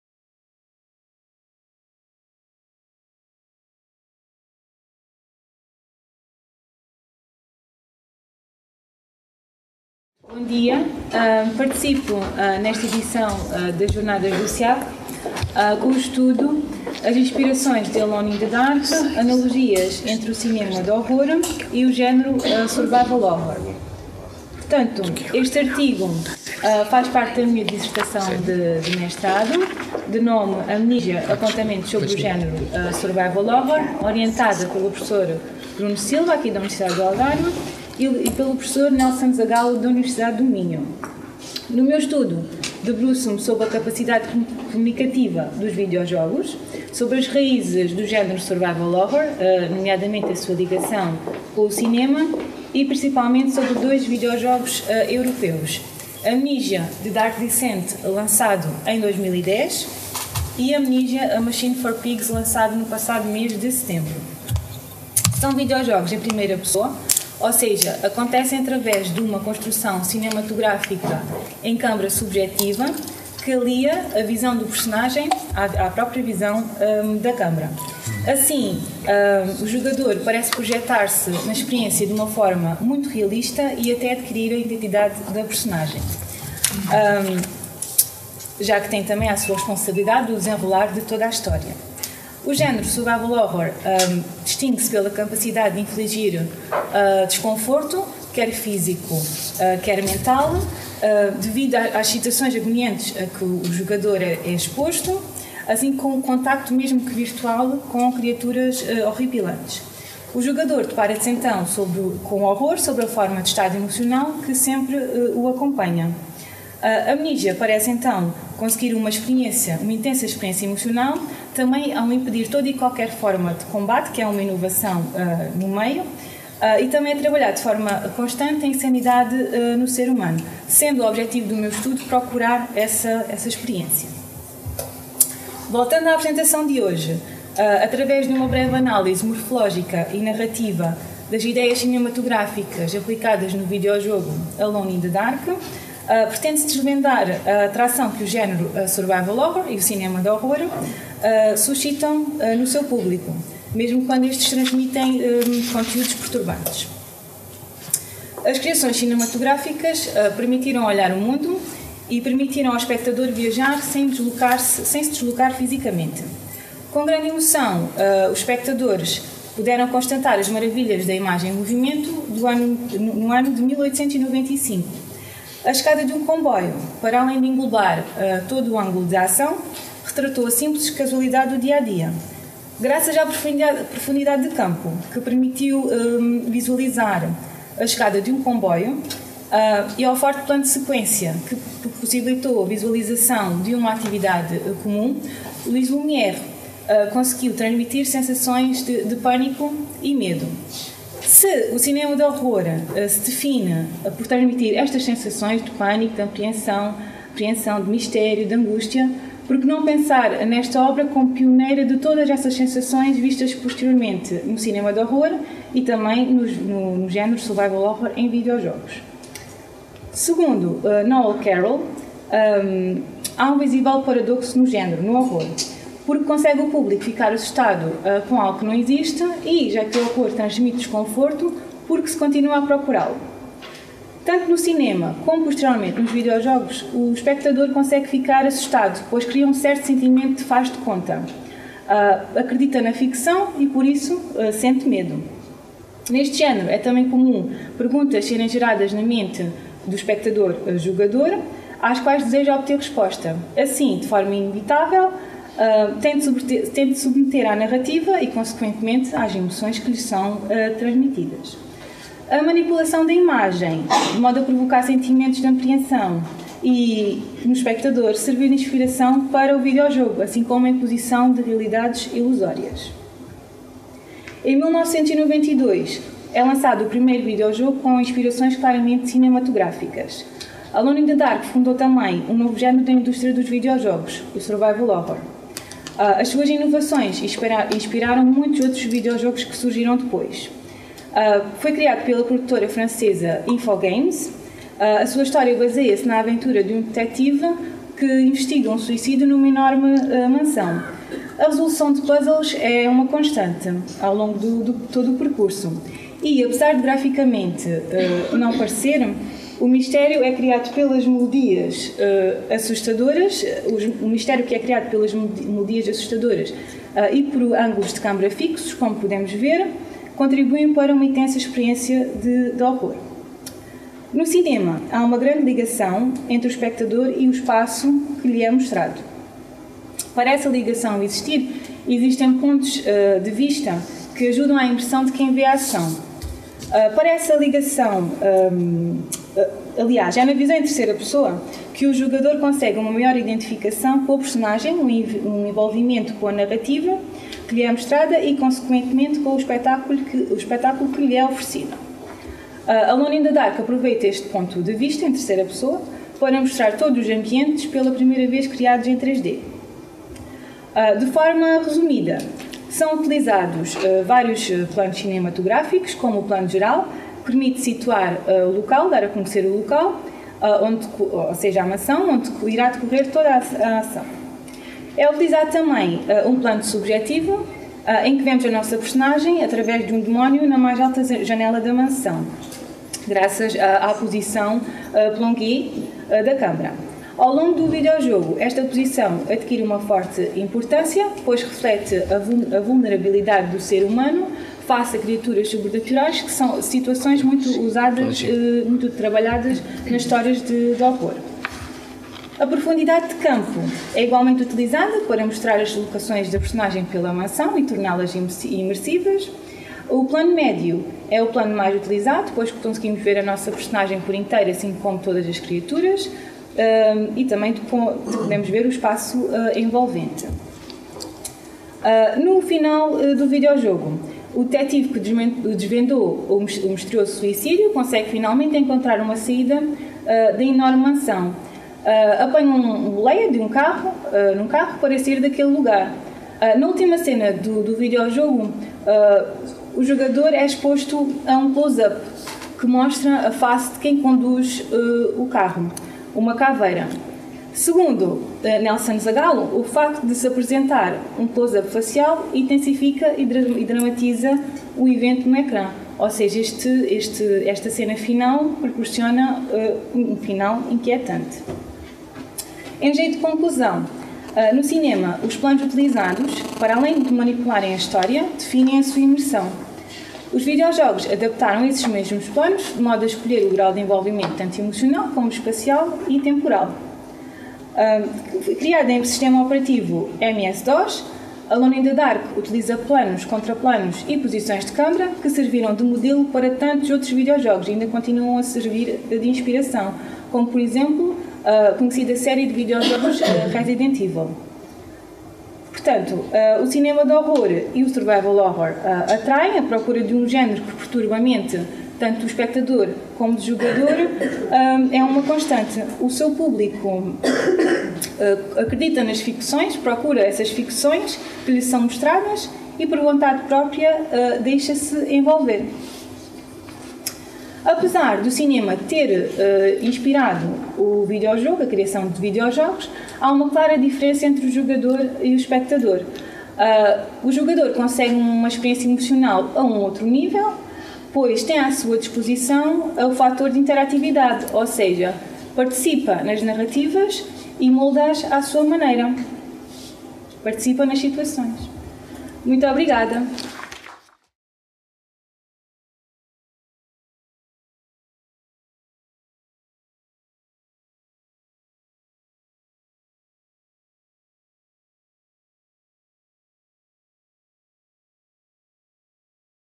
VII Jornadas de Investigação do CIAC | Ana Narciso
VII Jornadas de Investigação do CIAC  | As inspirações de Alone in the Dark -- Analogias entre o cinema de horror e o género survival horror | Ana Narciso

Licenciada em Ciências da Comunicação pela Universidade do Algarve, Ana Narciso frequenta atualmente o mestrado em Comunicação, Cultura e Artes pela mesma universidade. Por gostar de belezas visualmente artificiais, criadas pelo imaginário humano, foca o seu estudo no videojogo como meio digital.